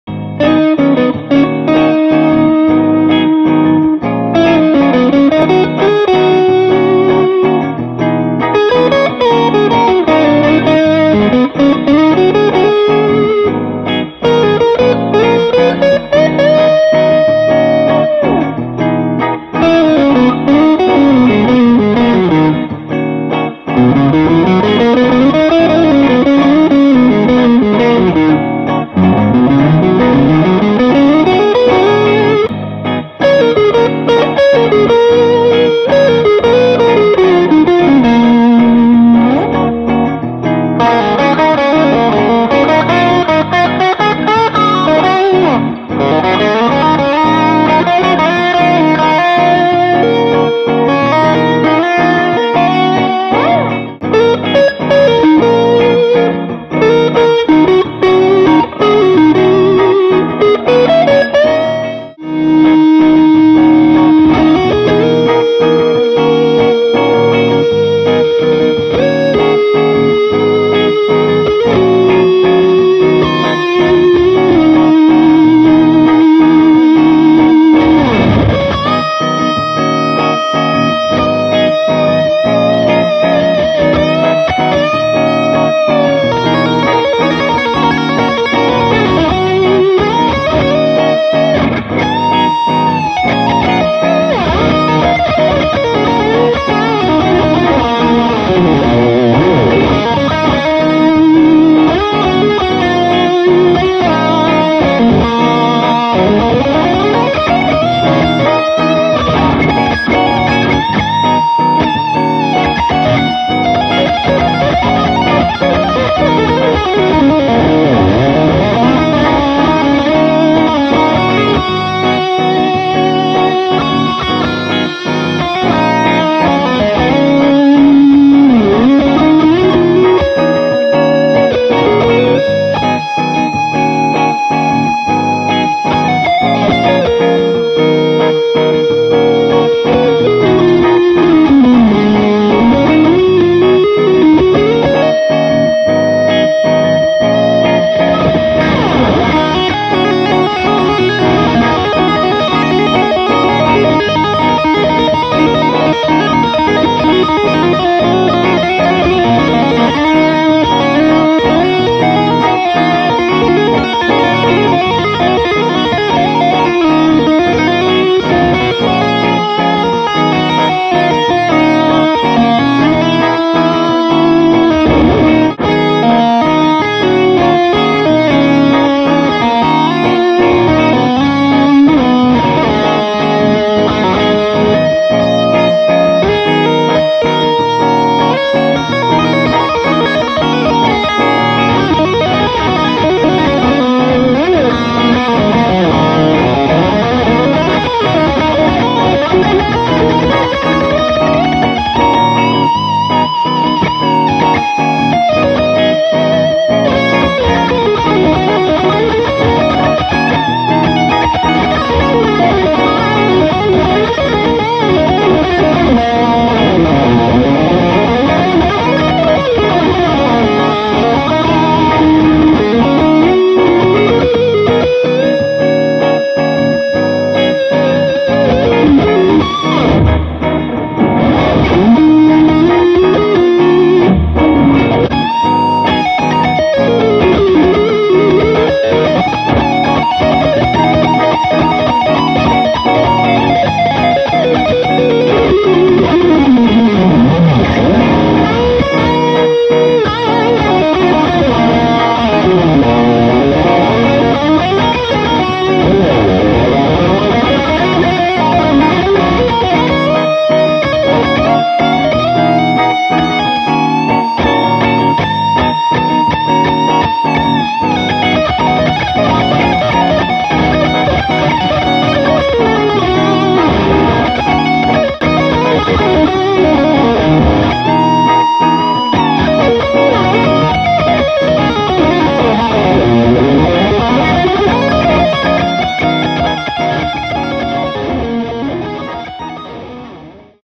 Gibson - Les Paul Special SL- Akai Head Rush looper jammin
This is a 1999 Les Paul Special SL (sans lacquer). This guitar has definitely got a laquer finish but I think they meant it like "no colors" were used, just a clear coat. Factory fitted with the original Gibson Humbuckers 498T / 490R, same pickups that are used for the Les Paul Custom.  This is a pure and lightweight rock guitar with a lot of punch, bite and sustain. It comes with mini trapezoid inlays and a belly shaping on the back. My bathroom scale says 3.2 kilos / 7.05 lbs but this puppy sounds like it weighed in at double. Credits and a huge Thank You to my friend Stephan.